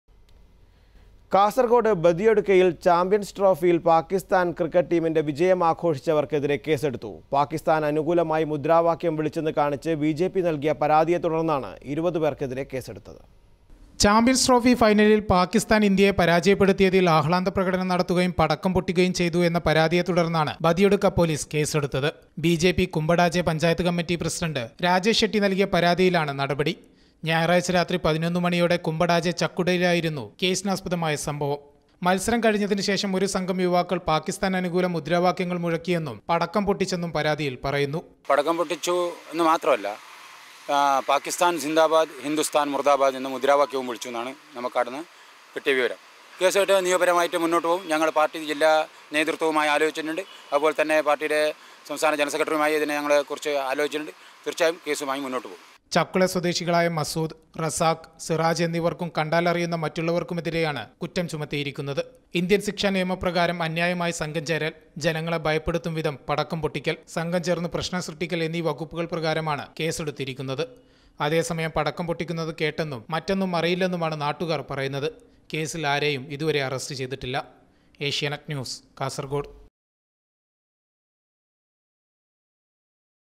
Case against 23 for celebrating Pakistan's victory
Badiyadukka police on Tuesday registered case against 23 people for celebrating the victory of Pakistan over India in the ICC Champions Trophy. BJP Kumbadaje panchayat committee president Rajesh  Shetty had in a petition stated that the people raised slogans against India and in favour of Pakistan on Sunday night. No arrests were made yet.